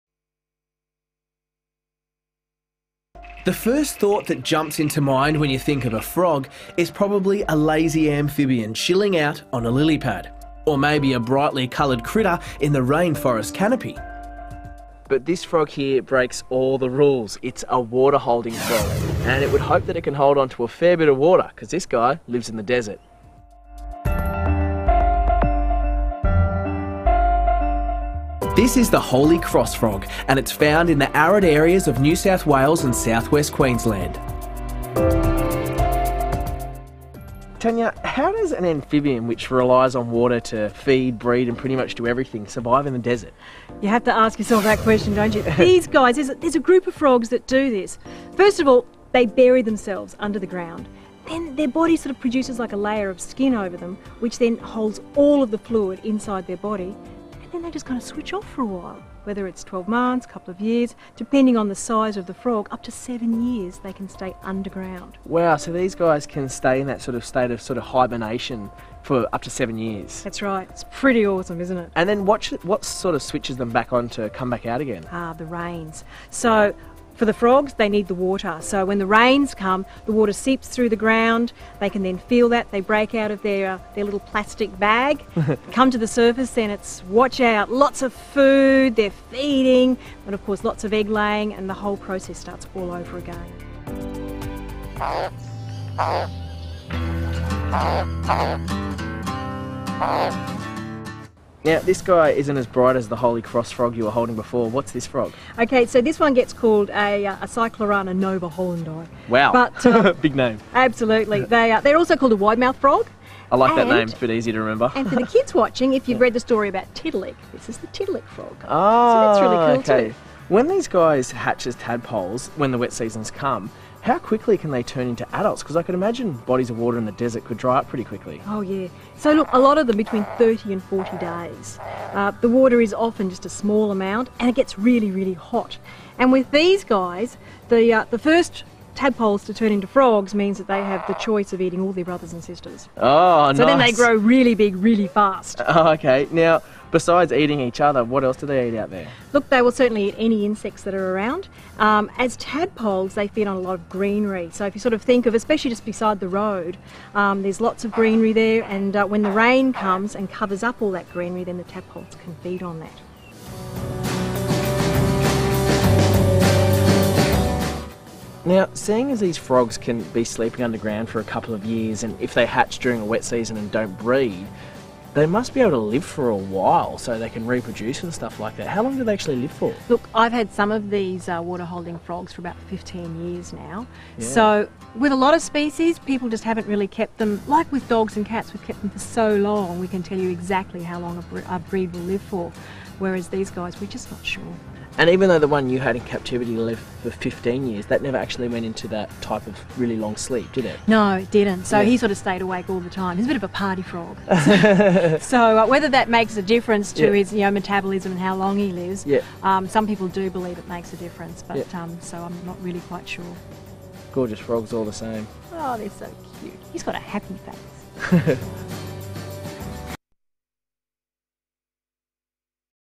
The Holy Cross Frog
The Holy Cross Frog. A Water holding frog that lives in the Australian dessert